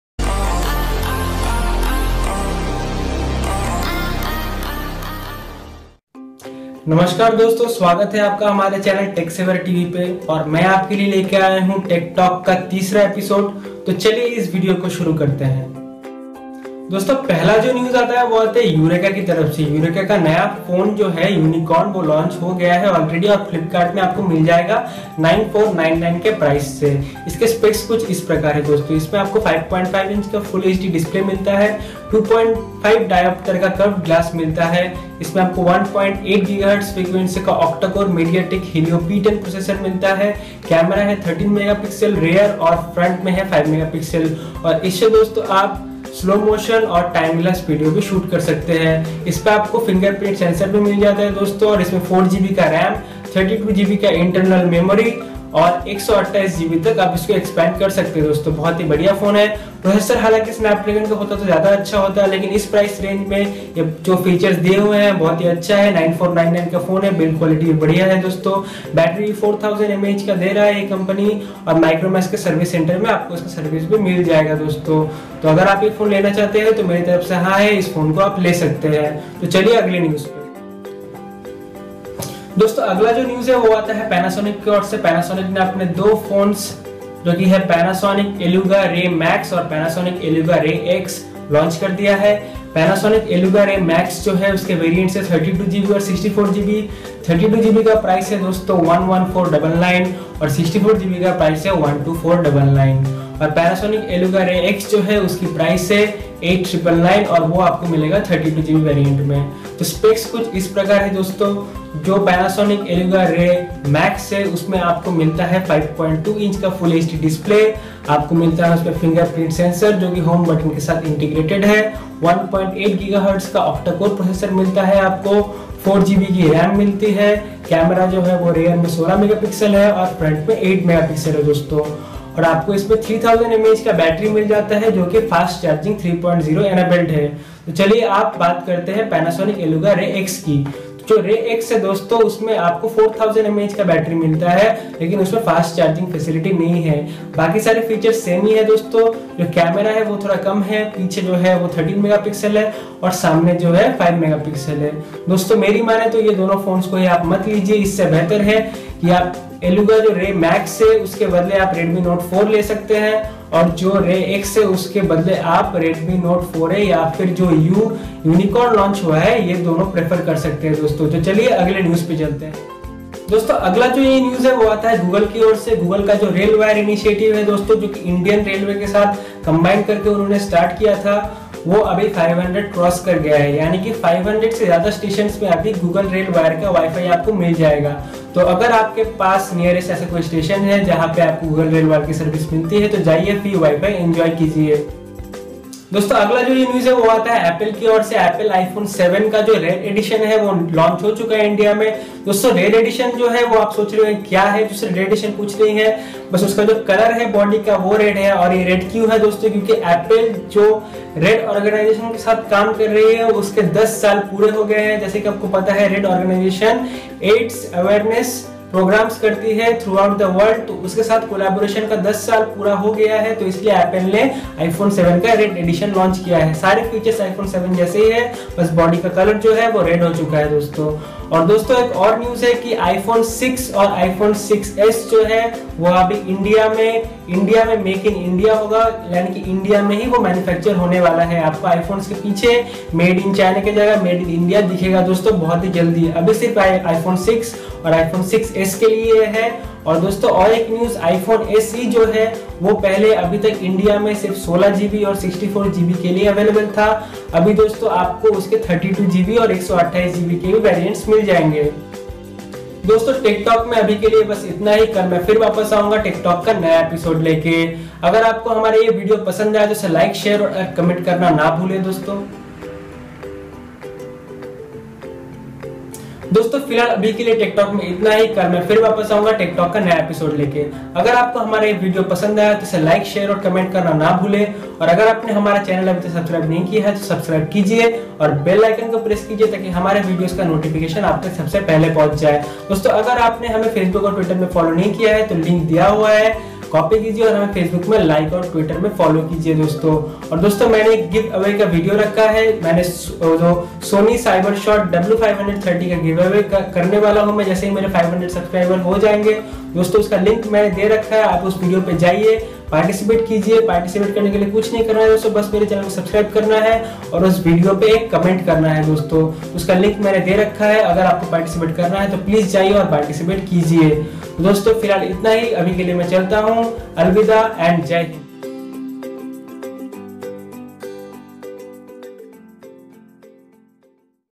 Tech talk #3 | Yu Yunicorn | Panasonic | RailWire | iPhone RED
Tech talk #3
Topics Discussed
1. Yu Yunicorn
2. Panasonic eluga ray max
     Panasonic eluga ray x
3. Google railwire
4. iPhone related updates

Available in Google Play

Keep watching Tech Saber TV©

Channel Owned by - Rahul Mahalanabis